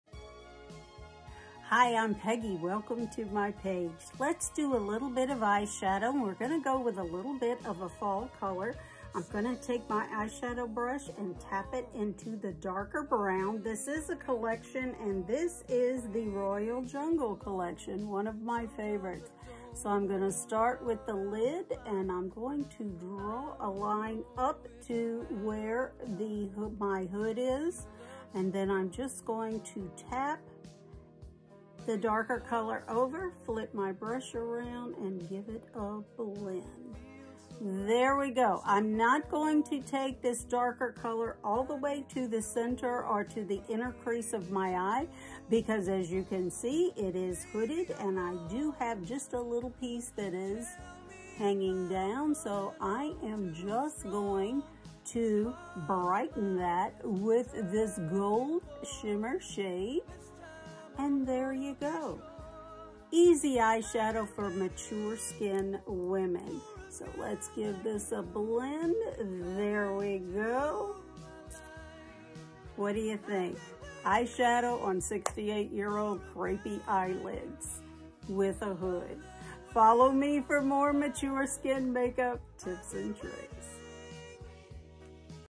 Mature Skin Makeup Over 60!
Celesty is a reputable European brand and their cream is a great option for ladies who couldn’t afford to start with a Seint Pallet, but still want to use a high quality cream makeup. Feedback from those who have bought and used Celesty has been incredible, 5 stars!!! 🌟 🌟🌟🌟🌟. Comment Beauty for Link! 🔗